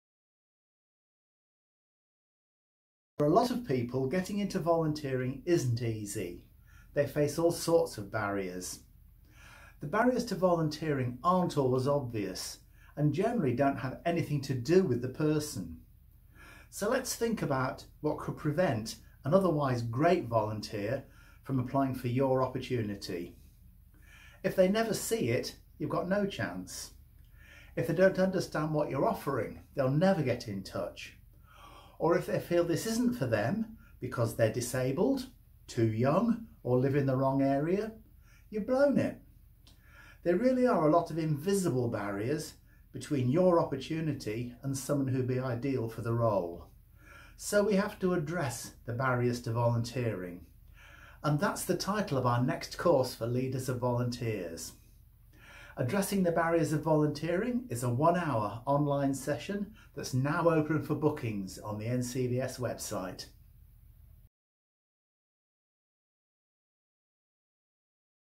NCVS course: Addressing the Barriers to Volunteering
In this one-hour online session, we will identify some of the barriers that potential volunteers face to becoming involved in your organisation and share ideas to remove these barriers.

This session will be helpful for those who are new to working with volunteers or those who want to improve or gain new ideas for their group's volunteer management processes, making it a useful refresher for more experienced leaders.

Course cost: £10

Find out more about available dates for this course and other training opportunities for Leaders of Volunteers by visiting our